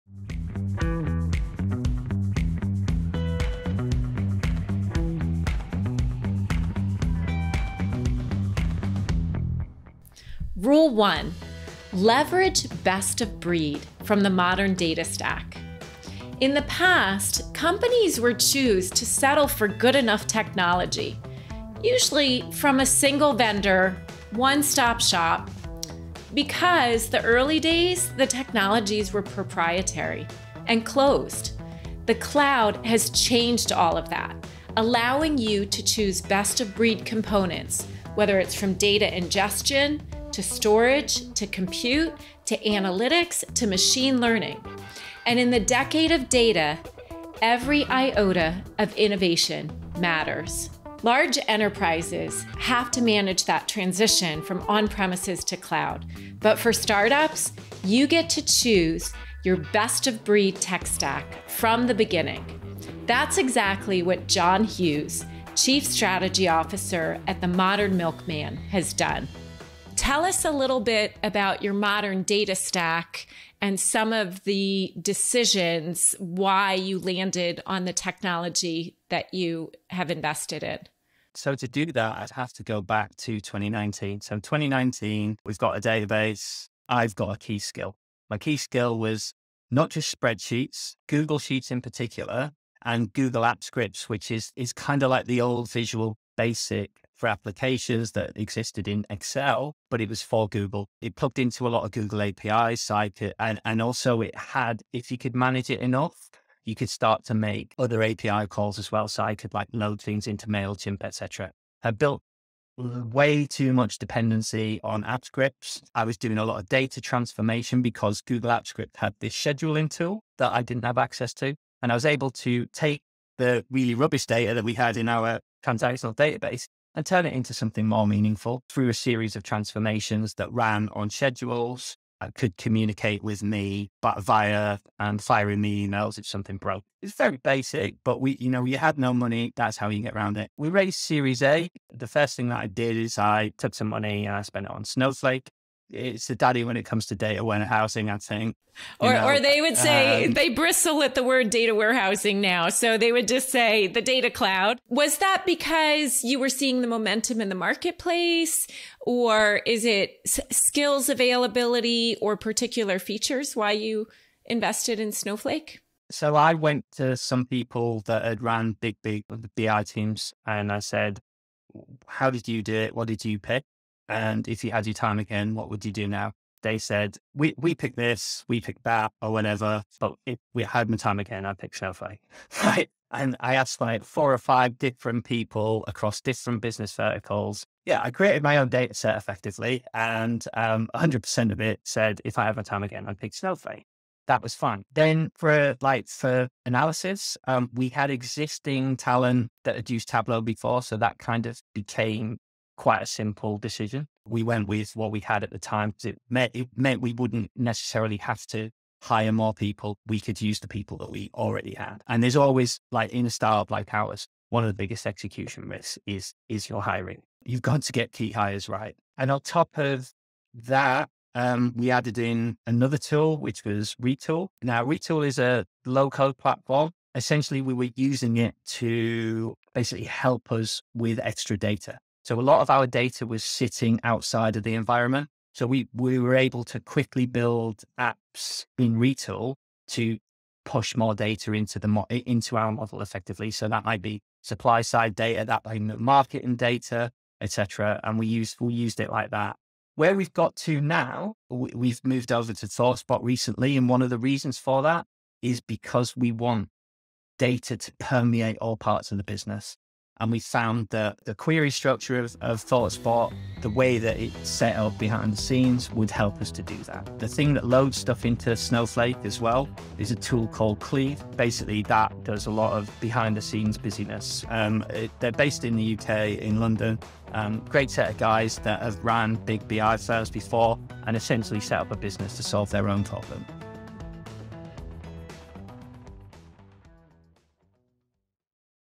Rule 1: Leverage the best-of-breed from the modern data stack
John Hughes, Chief Strategy Officer at The Modern Milkman, breaks down the company’s modern data stack and why startups have a modernization advantage over larger enterprises.

Get even more insights from data and analytics leaders like John on The Data Chief.

Mission.org is a media studio producing content for world-class clients.